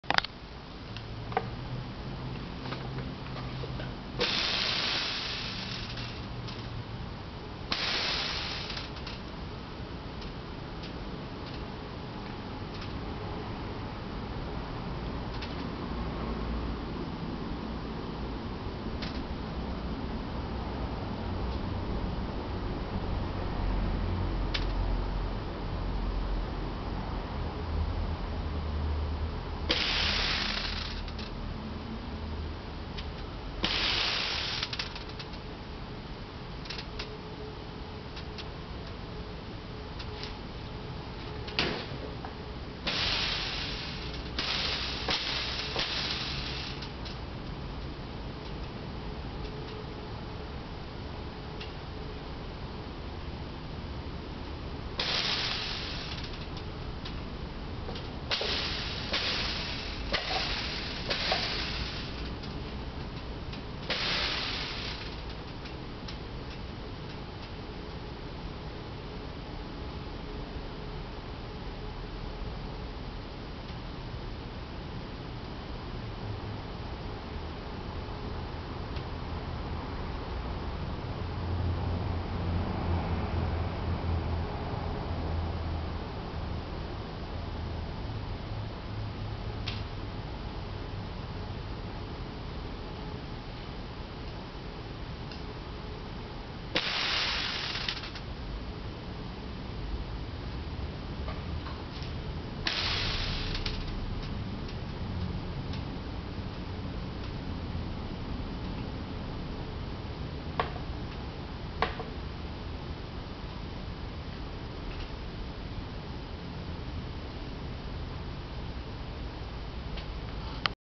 laser fence up and running
blowing fog machine at the laser fence i built for phage/the institute